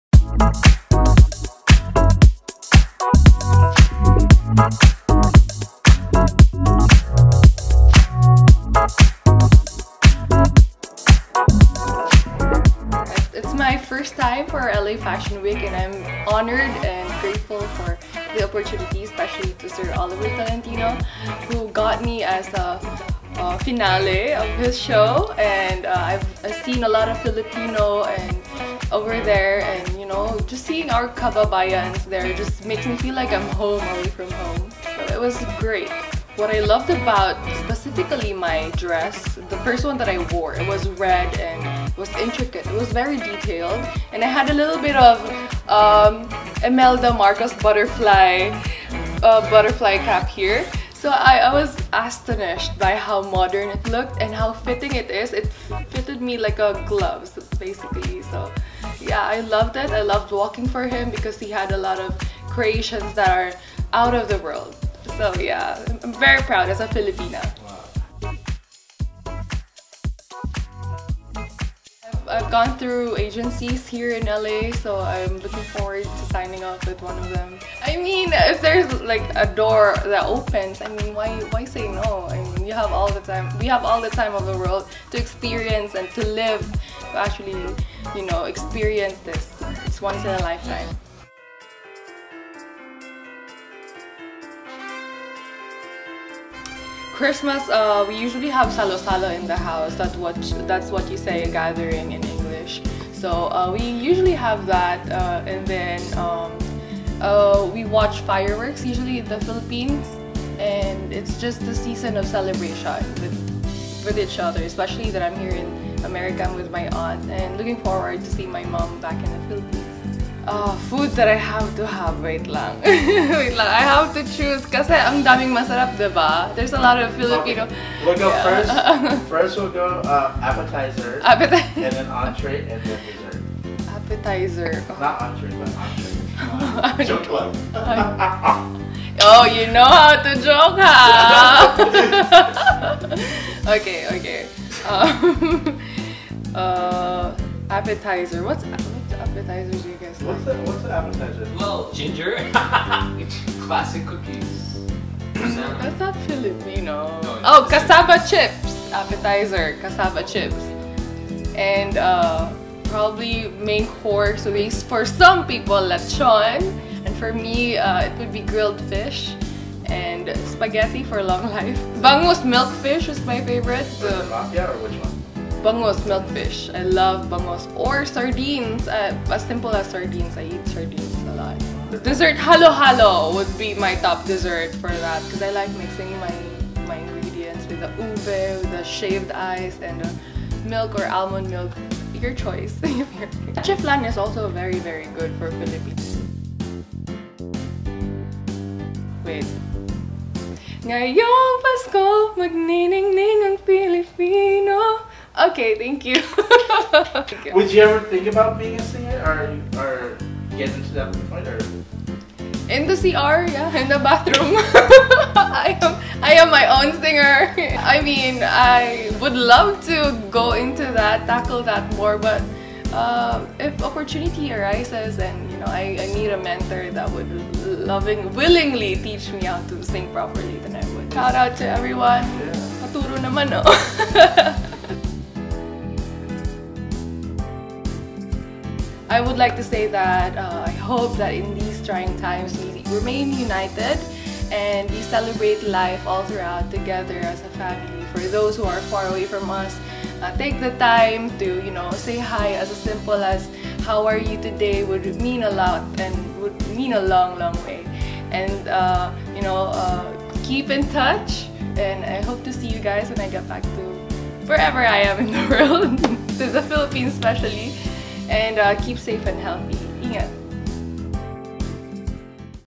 Gazini Ganados Talks LA Fashion Week Experience & How She Celebrates Christmas
Miss Universe Philippines 2019 Gazini Ganados took a moment out of her busy schedule while in LA to speak to Pacific Rim Video and talk about her recent runway experience at the LA Fashion Week walking for designer Oliver Tolentino and how she celebrates Christmas.

Clips featuring indviduals interviewed have verbally consented to be recorded and interviewed for editorial use.